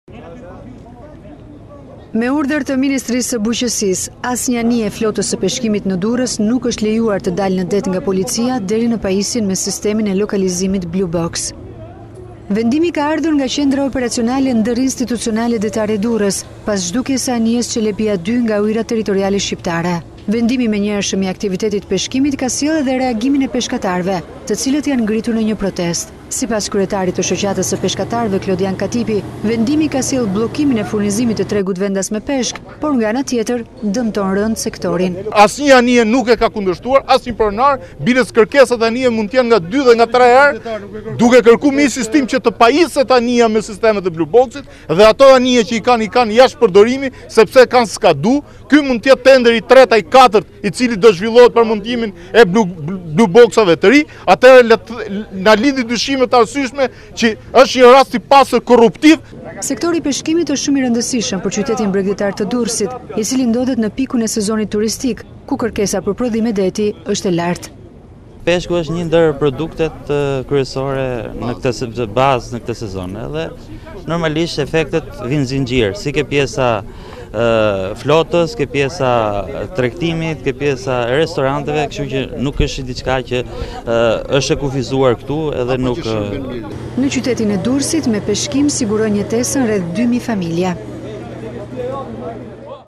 Ora News – S’lejohet të dalin në det 90% e anijeve, peshkatarët kundër vendimit të Ministrisë
Ministria e Bujqësisë ka urdhëruar mosdaljen në det të anijeve të peshkimit që nuk janë të pajisura me blue-box. Moslejimi i daljes së mjeteve lundruese që nuk kanë pajisjen sinjalizuese të vendndodhjes së tyre ka irrituar peshkatarët, të cilët janë ngritur në protestë kundër vendimit. Kjo masë vjen pas rrëmbimit të anijes së peshkimit “Celepia 2” nga një peshkatar egjiptian.
 Me urdhër të Ministrisë së Bujqësisë, asnjë anije e flotës së peshkimit në Durrës nuk është lejuar të dalë në det nga policia deri në pajisjen me sistemin e lokalizimit Blue-Box.
Vendimi ka ardhur nga Qendra Operacionale Ndërinstuticionale Detare Durrës pas zhdukjes se anija “Çelepia 2” nga ujërat territoriale shqiptare.
Bllokimi i menjëhershëm i aktivitetit të peshkimit ka sjellë dhe reagimin e peshkatarëve, të cilët janë ngritur në një protestë.
Sipas kryetarit të Shoqatës së Peshkatarëve, Klodian Katipi, vendimi ka sjellë bllokimin e furnizimit të tregut vendas me peshk, por nga ana tjetër dëmton rëndë sektorin.